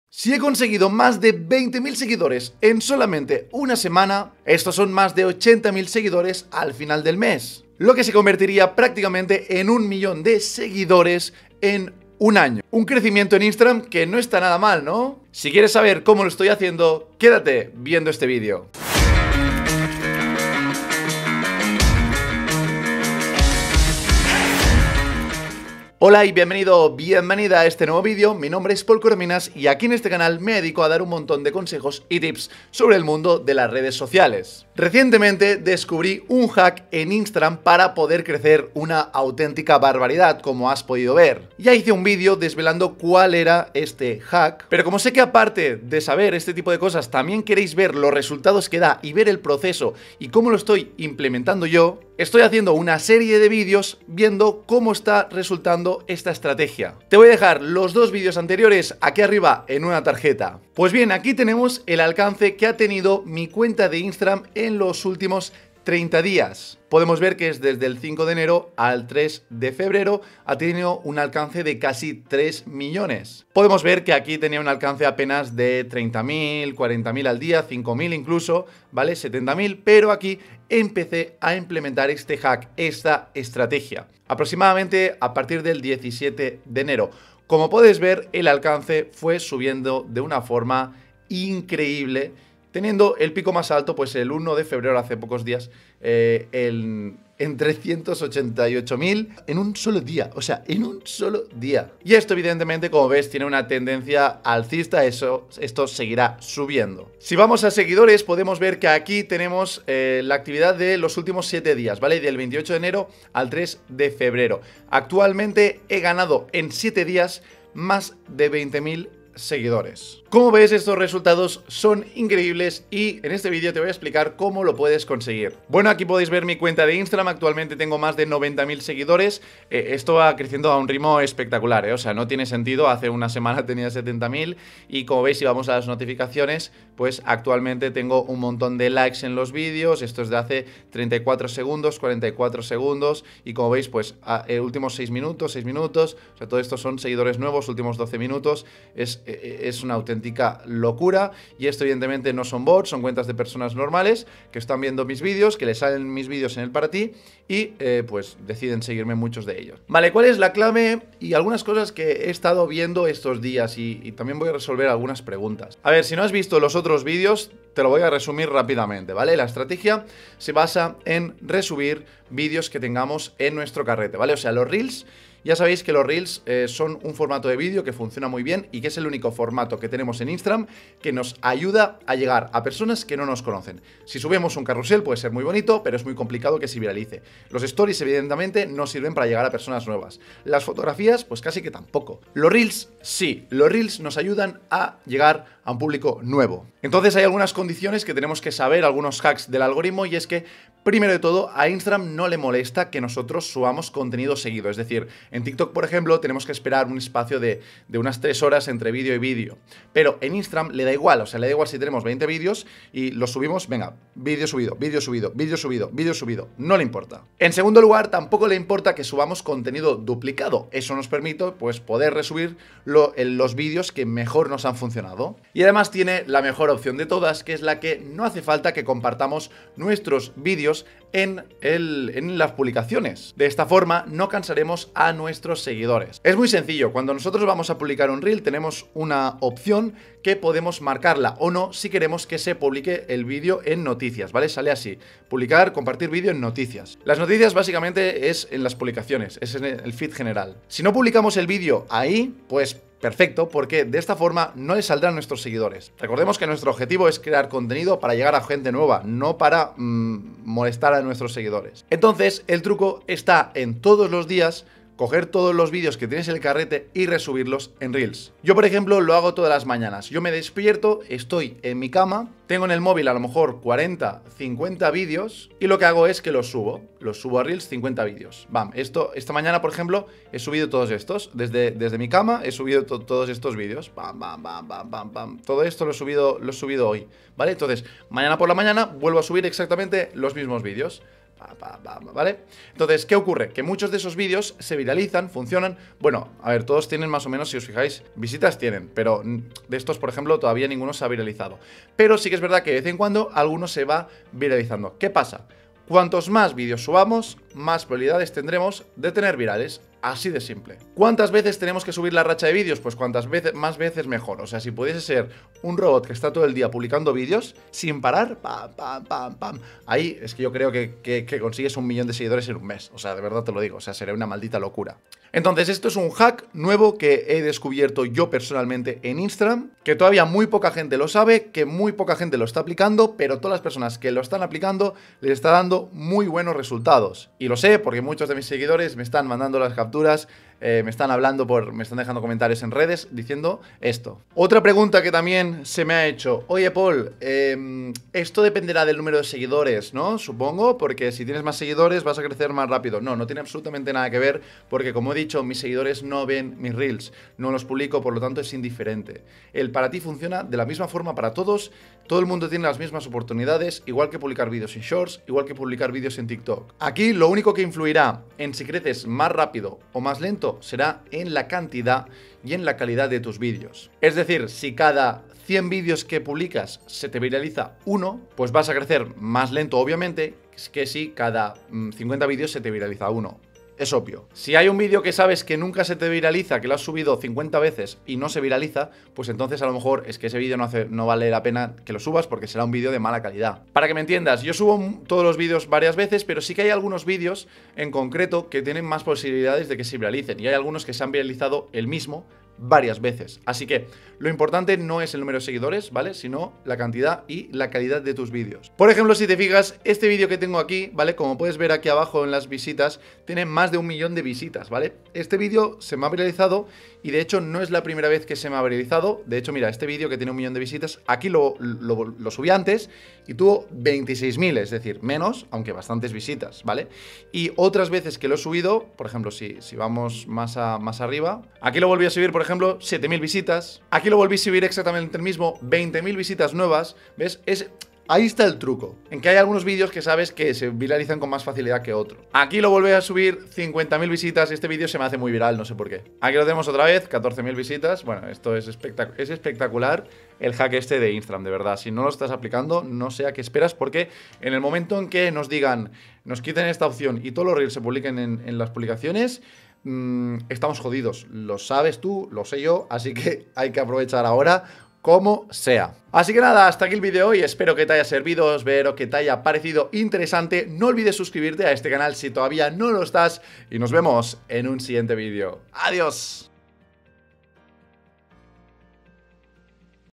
📢 Nuevo HACK para tener MÁS SEGUIDORES y LIKES en Instagram✅ 2022
Bienvenido a un nuevo vídeo de la serie de Instagram tips, una seríe de vídeos en las que te enseño a conseguir más seguidores, salir siempre en para ti, tener virales y ganar dinero gracias a subir vídeos en Instagram.

◕ ¿Cómo edito mis vídeos? ◕